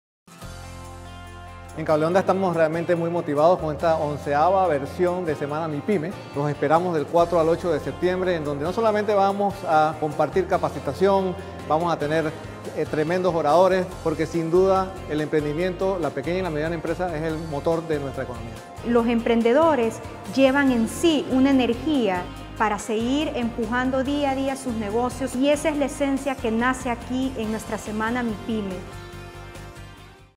¡Asiste a la 11ma Semana Mipyme!
SemanaMipyme2017 ¡conecta tu energía emprendendora y dale luz a tus ideas! y obtén consejos de los emprendedores más exitosos de Panamá!